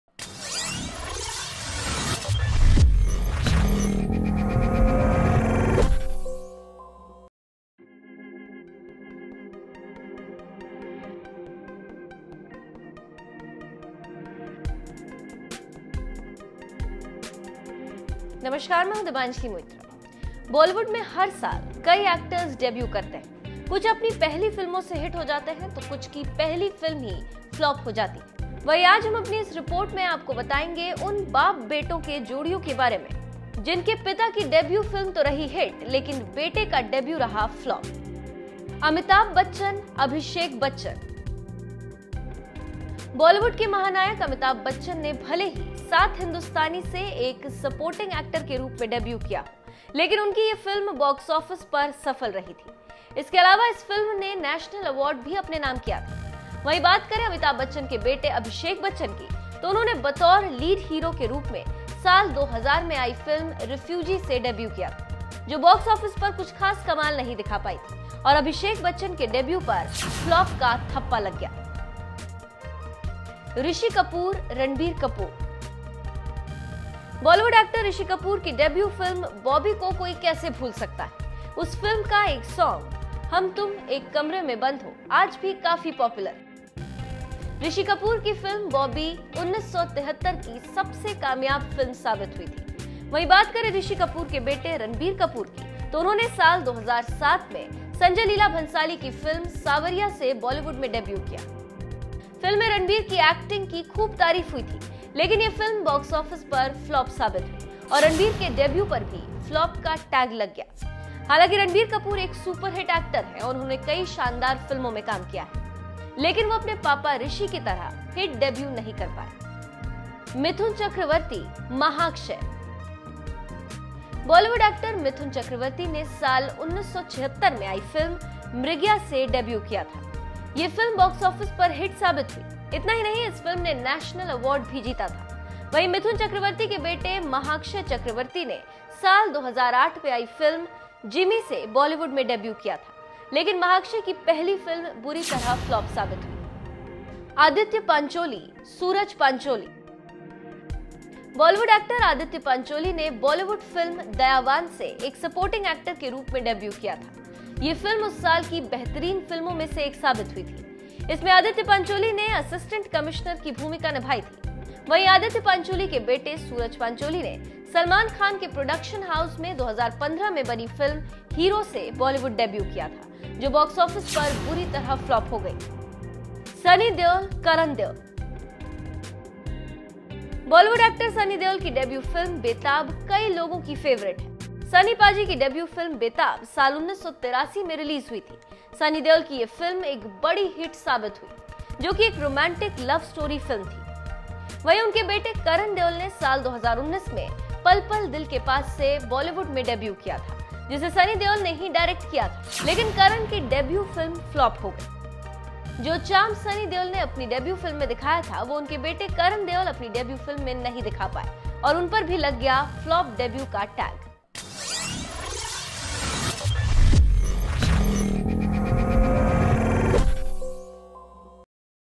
बाप की Debut Film हुईं Hit और बेटों की Debut Film रहीं Super Flop
बॉलीवुड में हर साल कई एक्टर्स डेब्यू करते हैं, कुछ अपनी पहली फिल्मों से हिट हो जाते हैं तो कुछ की पहली फिल्म ही Flop हो जाती है. वहीं आज हम अपनी इस रिपोट में आपको बताएंगे की उन बाप बेटों की जोड़ियों के बारे में जिनके पिता की डेब्यू फिल्म तो रही हिट, लेकिन बेटे का डेब्यू रहा Flop...